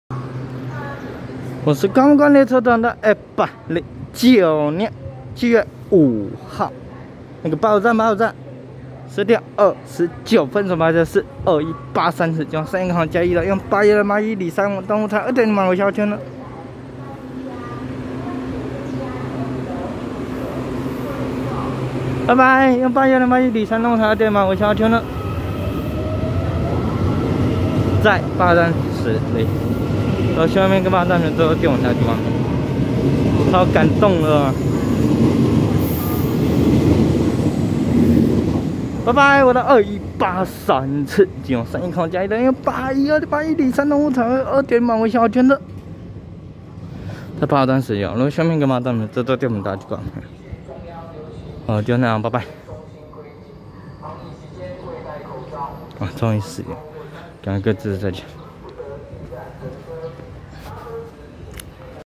2183次經由山線開往嘉義的EMU812+EMU811里山動物彩繪2.0版微笑號區間車
109/7/5八堵站10:29分拍攝2183次經由山線開往嘉義的EMU812+EMU811里山動物彩繪2.0版微笑號區間車